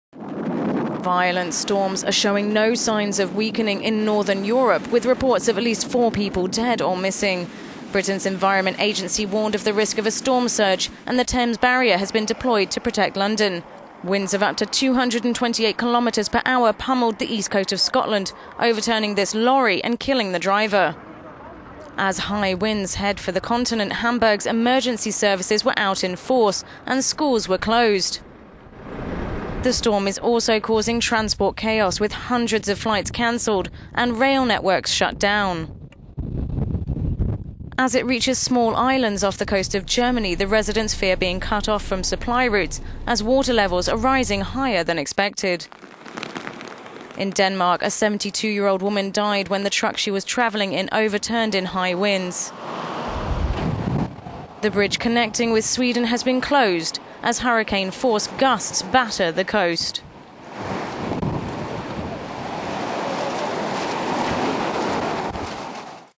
Storm continues to leave trail of destruction in Europe
Violent storms are showing no signs of weakening in Northern Europe, with reports of at least four...

Violent storms are showing no signs of weakening in Northern Europe, with reports of at least four people dead or missing.

Britain's Environment Agency warned of the risk of a storm surge along the coast leading to evacuations and the Thames barrier being deployed to protect London.

Winds of up to 228 kph pummeled the east of Scotland, overturning a lorry near Edinburgh and killing the driver.

As high winds head for the continent fallen trees littered roads. Hamburg's emergency services were out in force to deal with the debris and schools were closed.

The storm is also causing transport chaos with hundreds of flights cancelled and rail networks shut down.

As it reaches small islands off the coast of Germany the residents fear being cut off from supply routes as water levels are rising higher than expected.